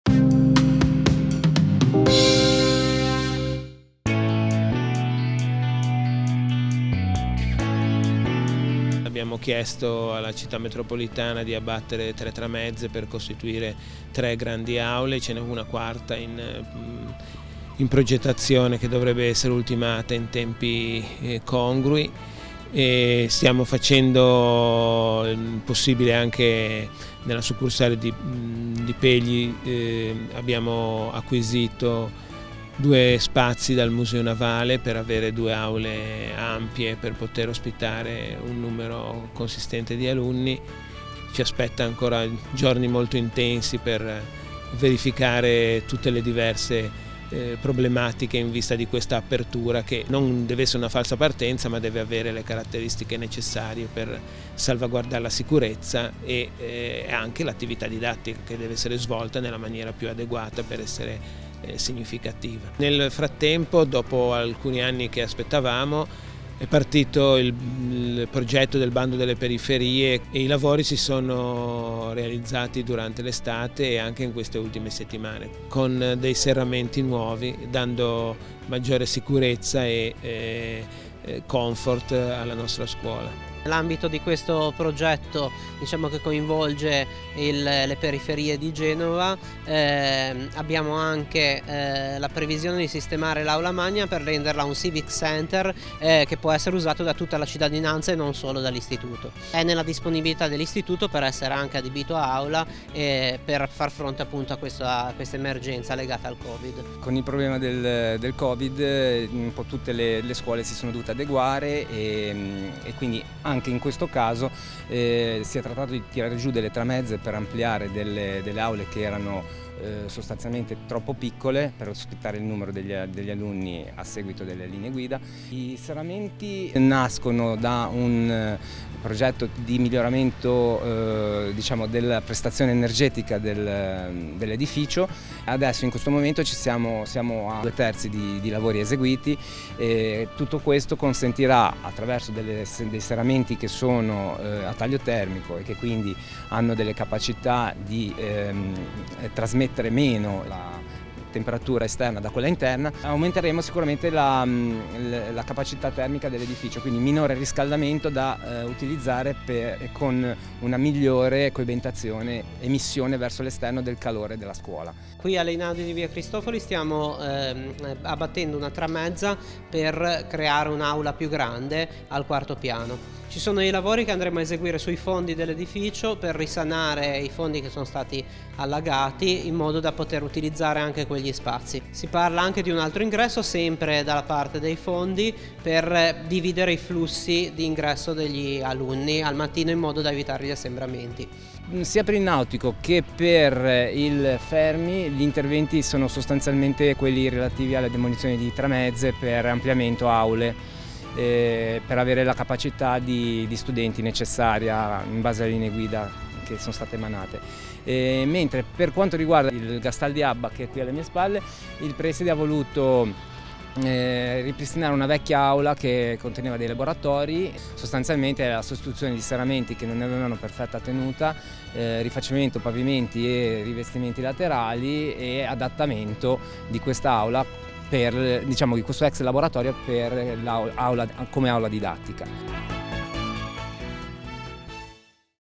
Scuole superiori Centro-Ovest: distanziamento e spazi futuri per la rigenerazione di quartiere
La maggioranza degli istituti superiori è in questi giorni nelle fasi finali dell’adeguamento degli spazi didattici, come previsto dalle misure ministeriali anti Covid.
In questo video, insieme ai tecnici della Città Metropolitana di Genova e al sub Commissario delegato ai lavori, l’arch. Davide Nari, durante un sopralluogo in alcuni plessi del Centro Ovest, abbiamo incontrato il dirigente del Liceo Classico e Linguistico G. Mazzinii Mario Eugenio Predieri che ci ha illustrato come il Liceo, sia nella sede che nella succursale di Pegli, abbia potuto trovare gli spazi per il corretto distanziamento tra gli allievi, questo abbattendo tramezze tra aule adiacenti, con la collaborazione dell’ente metropolitano, riutilizzando spazi altrimenti destinati ed anche occupando temporaneamente locali di altre istituzioni.
Analogamente, potranno essere temporaneamente usati allo scopo gli spazi che il cosiddetto “Bando periferie” ha rigenerato con finalità di Civic Center, mentre sono ancora in corso i lavori di miglioramento delle prestazioni energetiche con la sostituzione dei vecchi serramenti con i nuovi a taglio termico.
Diverse le tramezze abbattute anche all’Einaudi di via Cristofoli, che ha contestualmente visto il ripristino di spazi nei fondi dell’edificio, da tempo inutilizzati a causa di allagamenti, e la progettazione di un nuovo ingresso laterale per regolare il flusso degli studenti.
Molti anche i lavori nel vero e proprio polo della formazione in via Dino Col, dove trovano posto diversi istituti, come il Gastaldi che ha ricavato spazi da vecchi laboratori rifacendo, con la collaborazione di Città Metropolitana, la pavimentazione ed i serramenti di tali ambienti, o come l’istituto Nautico S.Giorgio ed il liceo sceintifico Fermi che per le loro succursali e che hanno optato per l’abbattimento di tramezze al fine di aumentare i volumi a disposizione degli studenti e garantire nei tempi previsti la riapertura dei plessi.

Città Metropolitana di Genova
Sottotitoli a cura di Livio Marciano